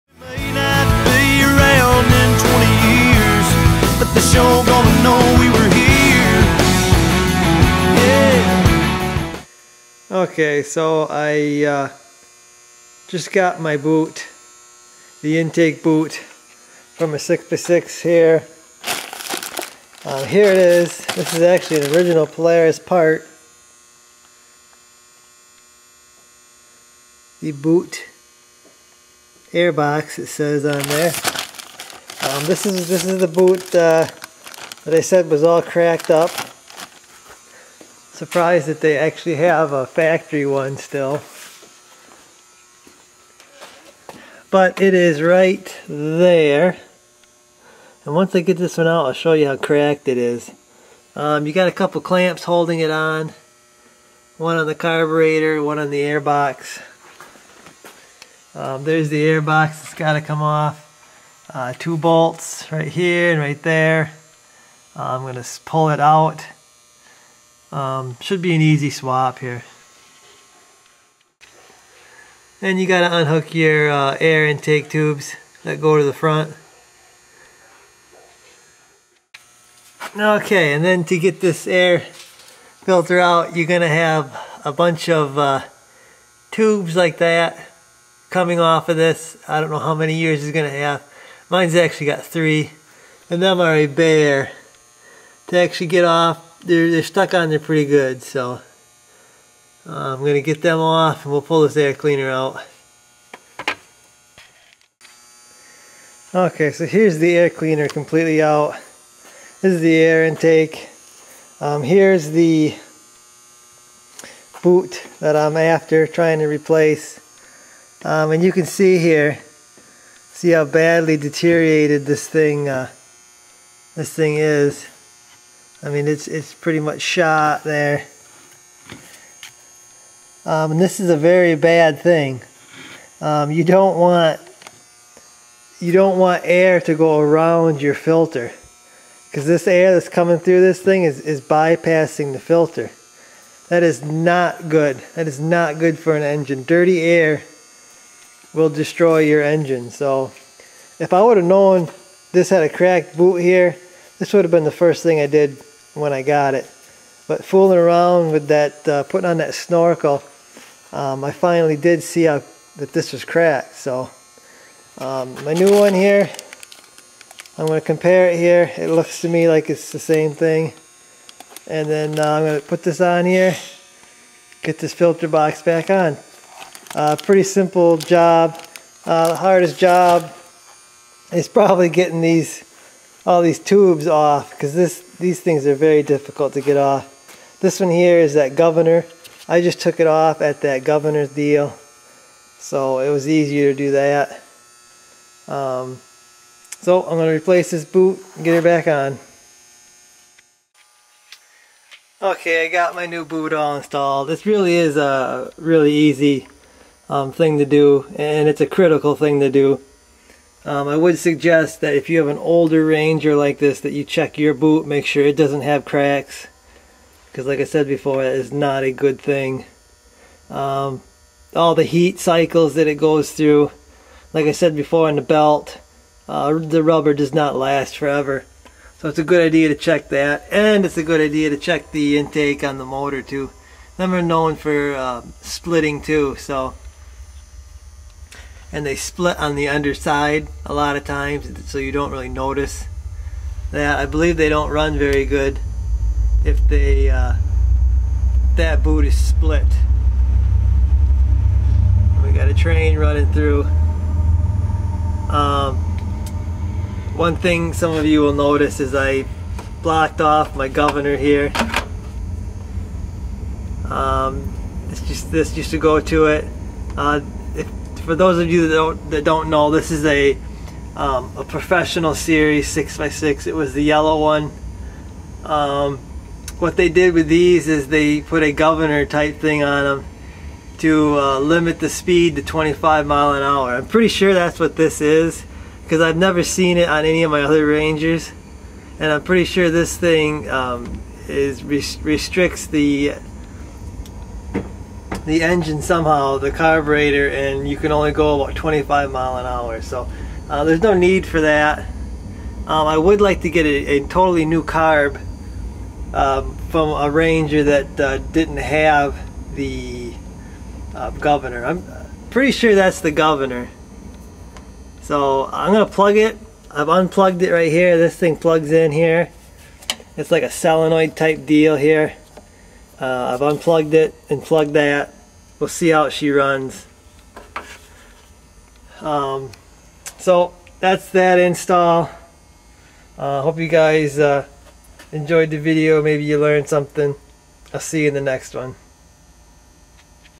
2003 Polaris Ranger 6x6 "Intake Boot Replacement"
My carb to air box boot was cracked so I replaced it.  Very easy and important job.  You don't want unfiltered air to go into your engine.